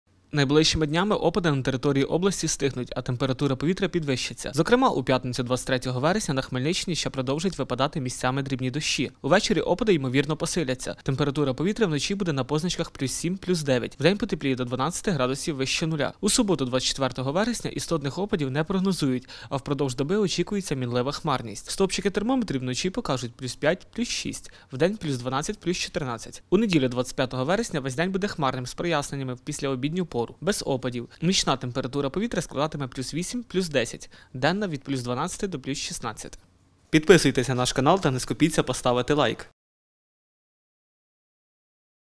Прогноз погоди на тиждень 23 - 25 вересня 2022 року у Хмельницькій області від Є ye.ua
Найближчими днями опади на території області стихнуть, а температура повітря підвищиться.